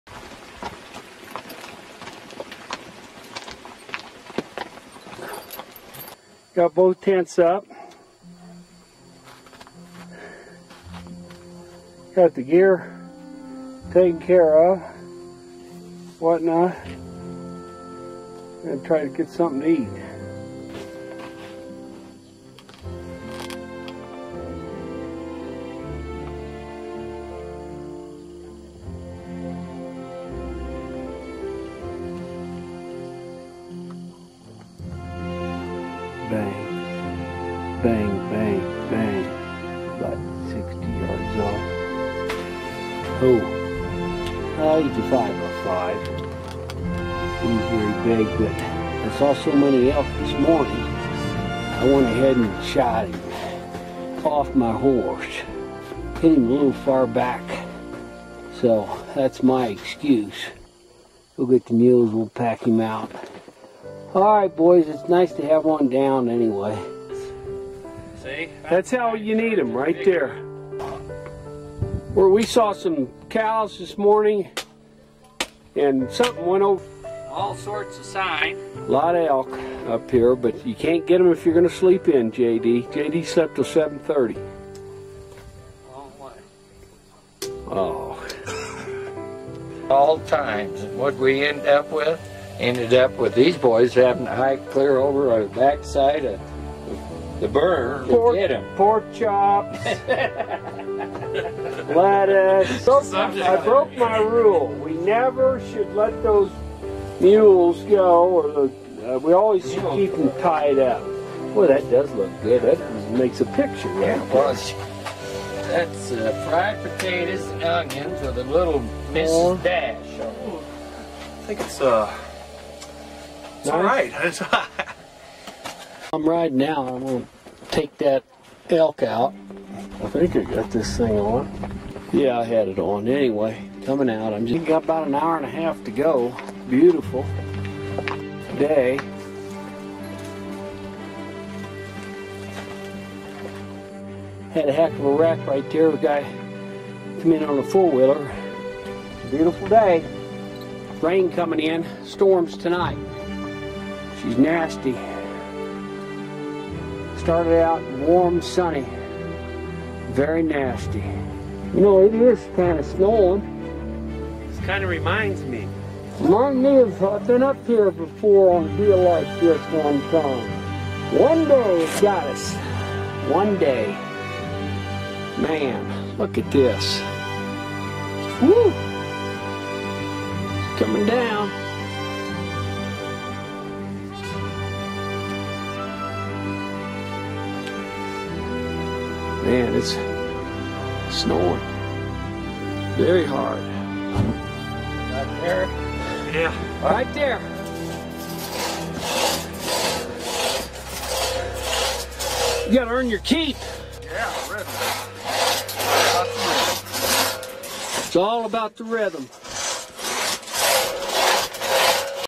Final with music elk shot off horse black white
A-Wall tents, pack mules and horses 18 miles into the wilderness. a 6-hour horseback ride to camp. deep in the Weminuche. camped at 11,900 ft elevation. in a snowstorm...

Do-It-Yourself elk hunts. there ain't nothing better. invigorates the soul. a brain reset, so to speak. we like camping high because in October...the mosquitoes ain't too bad, that high. and we can get above the elk.

lots of fellas tell me, "I can ride a horse" ...We ain't worried about that. what a DIY elk camp wants to know is , "How are you on the end of a two-man cross-cut saw. How's about swinging a 6-lb splitting axe. How much firewood can you haul in two trips. Can you haul a 5-gallon bucket of water 200-yds, up hill. And what kind of shape is your heart in".

in 45-years of elk hunting, I've shot an elk off my horse a total of one time. in fact, this is the only time I ever even tried to shoot an elk off a horse or mule. on this hunting trip. it's a long story, but I ain't planning on trying it again.

But there ain't nothing better than a wilderness elk hunting trip...the one fellow in this video...my good friend J.D...he died of liver cancer a few years after this trip. many good times we had, hunting elk in the mountains. you don't live forever...

as for elk hunting, generally the early bird gets the worm. nobody has ever shot an elk inside the tent. it helps to be as brave as Caesar...find somebody with elk hunting experience, and stick to him like a bur to sheep’s wool. it is mainly a young man's sport, but for the older fellas, slow and steady wins the race...and it snows in Colorado in October